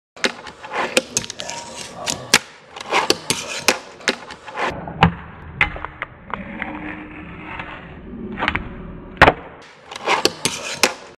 i lost o well
i lost so bad this trick is so sketch compared to tim o well
i felt like posting this cuz i was bored and i want to get on ffi.
ive been fbing for over a year now and i think i deserve it. lol.
i figure posting responses to vids will get my name out.
peace.
o and i no this doesnt count lulz